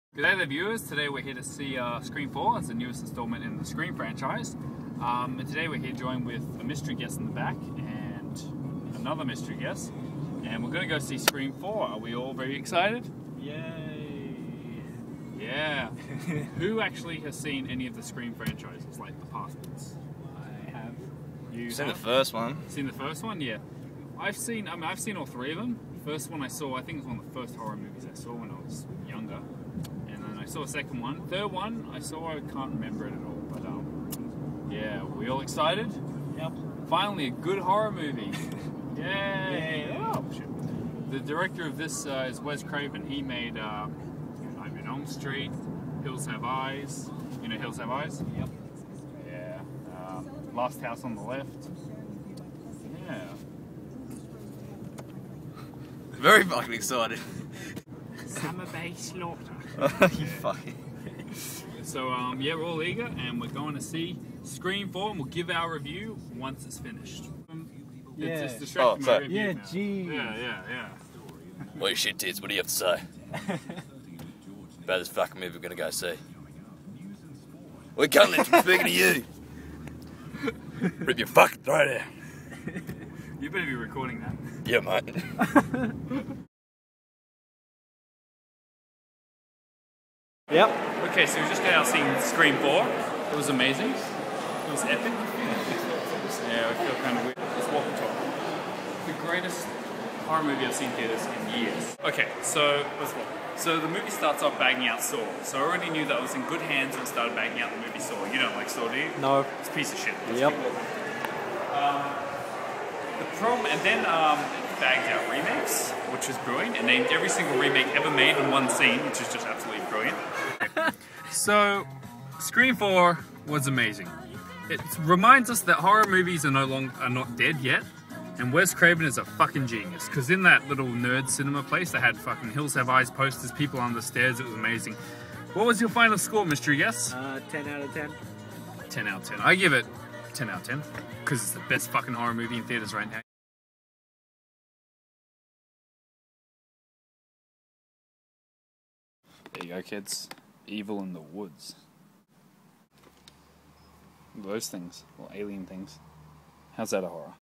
Seeing Scream 4
Our Very Quick review on scream 4, this is a very fast paced video on my opinion of wes cravens new scream film.